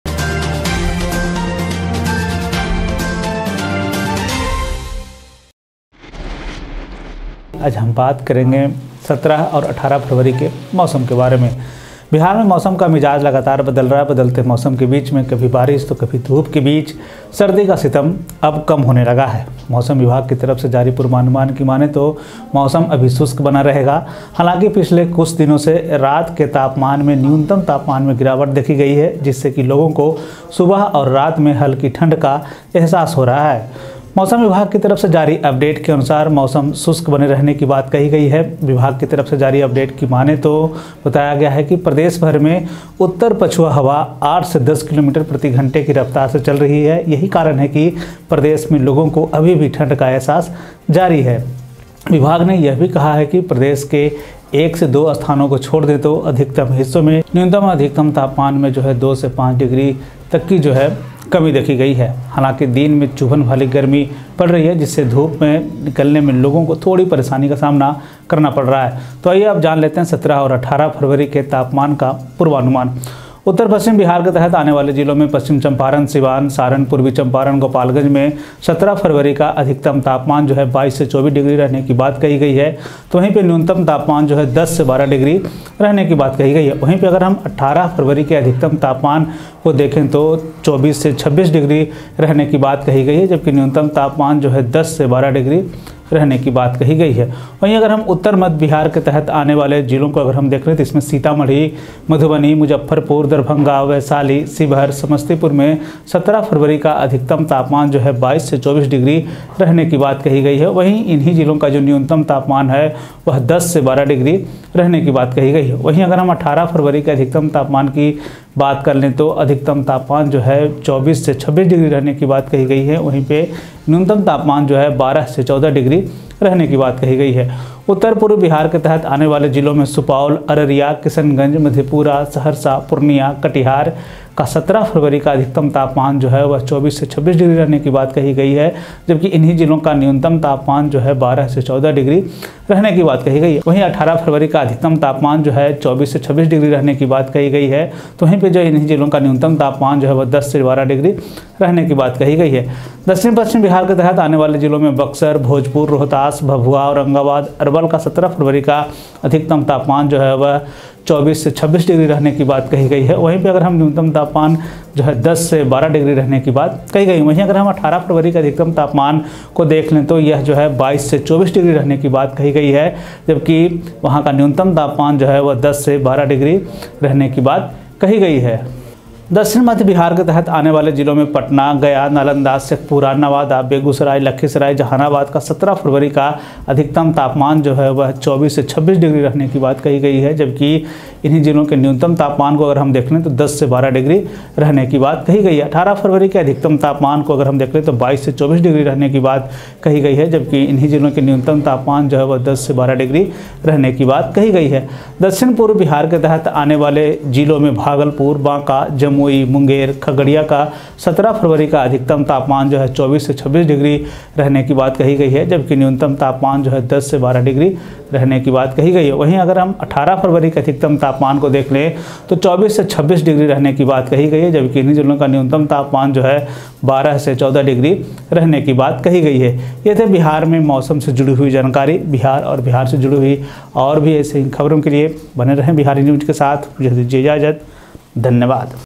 17,18 फरवरी का बिहार मौसम अपडेट.Bihar weather update of 17th and 18th February 2022.
17,18  फरवरी का बिहार मौसम मौसम अपडेट.Bihar weather update of 17th and 18th February 2022.

      Among the districts falling under north-west Bihar, west Champaran, Siwan, Saran, East Champaran, Gopalganj  have been predicted to have  a maximum temperature  of 22 to 24 degrees for February 17, while the minimum temperature will be 10 to 12 degrees Celsius.  The degree has been said to be there while the minimum temperature is said  to  be 10 to 12 degrees.

If we talk about north central Bihar, then among the districts covered under it, sitamarhi, Madhubani, Muzaffarpur, Darbhanga, Vaishali, Sheohar, Samastipur  recorded    a maximum temperature of 22 to 24 degrees on February 17, while the minimum temperature was 10 to 12 degrees.  Talking about the maximum temperature of February, the maximum temperature  has been said to be 24 to 26 degrees, while the minimum temperature is  said to be  12  to 14 degrees.

Supaul, Araria, Kishanganj, Madhepura, Saharsa, Purnea, Katihar  recorded a maximum temperature of 24 to 26 degrees  celsius on February 17, while the minimum temperature of these districts  was 12 to  14 degrees, while the maximum temperature    of February 18 was 12 to 14 degrees.   The temperature  has been said to be 24 to 26 degrees, while the minimum temperature is   said to   be 10 to 12 degrees.

Among the districts falling under south-west Bihar, Buxar, Bhojpur, Rohtas, Bhabhua, Aurangabad, Arwal  have been predicted to have  a maximum temperature  of 24-26 degrees for February 17, while the minimum temperature was 10 to 112 degrees, while the  maximum temperature on February 18 was 22-24 degrees.  While the minimum temperature is said to be 10 to  12 degrees, the minimum temperature  is said to be 10 to 12 degrees.

Among the districts falling  under south central Bihar, Patna, Gaya, Nalanda,  Sheikhpura, Nawada, Begusarai, Lakhisarai, Jehanabad  have been predicted to have  a maximum temperature  of 24-26 degrees for February 17 while the minimum temperature will be 10 to 12 degrees.    It has been said to be 22 to 24 degrees, while the minimum temperature is said to be 10 to 12 degrees.